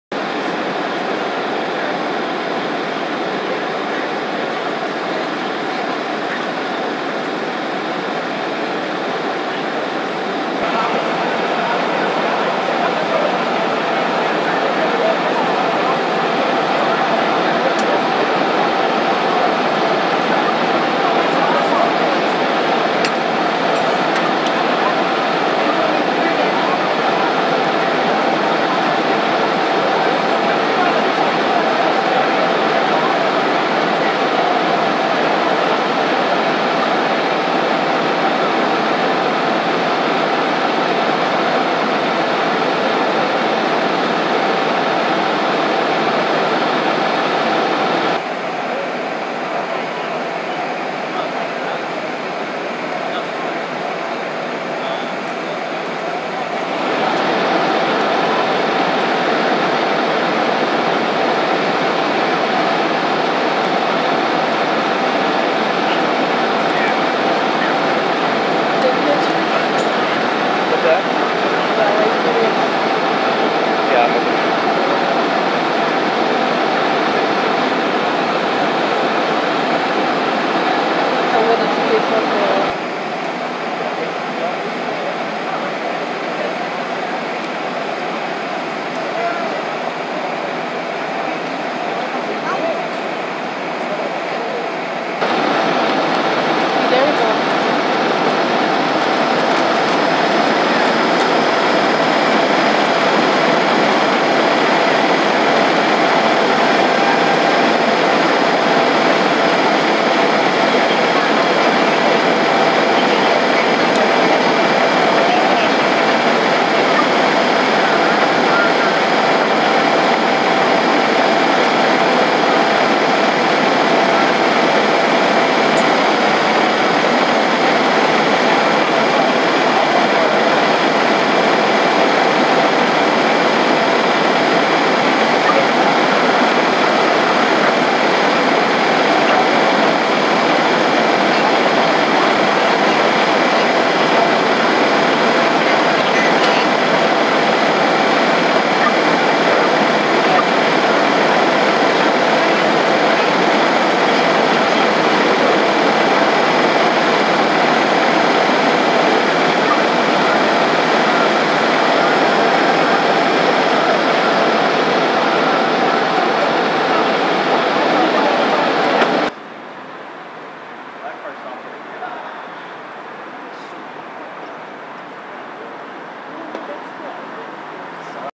1/1/18 Church Auto v Ped
LAFD responds to an Auto vs Pedestrian collision outside of a church